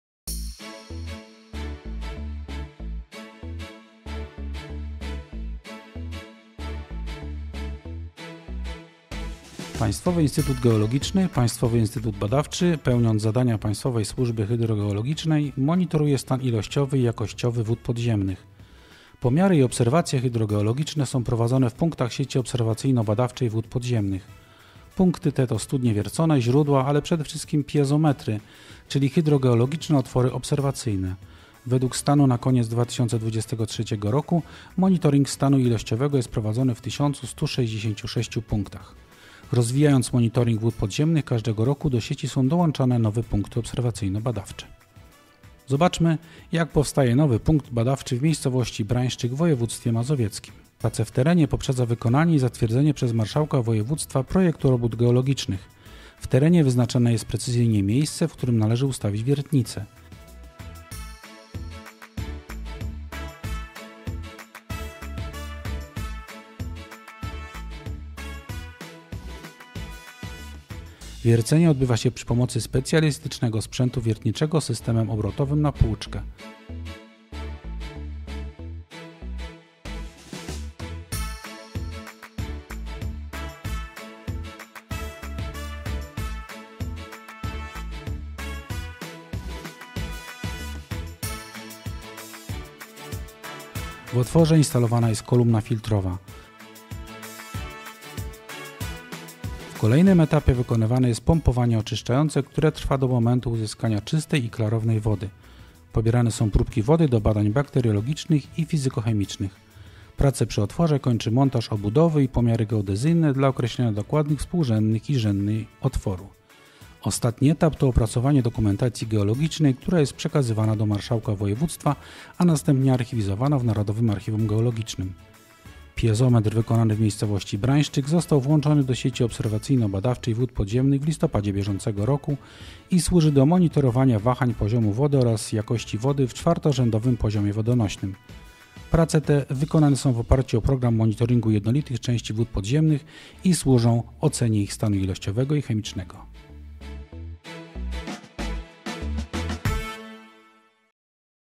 Rozbudowa sieci badawczo - obserwacyjnej wód podziemnych
Od pięciu dekad Państwowy Instytut Geologiczny monitoruje wody podziemne. Dzięki badaniom i obserwacjom w prawie 1900 punktach badawczych wiemy, jaki jest stan wód podziemnych w naszym kraju – zarówno ilościowy, jak i jakościowy, jak również możemy prognozować jego zmiany i szacować dostępność wód na potrzeby społeczeństwa i gospodarki kraju.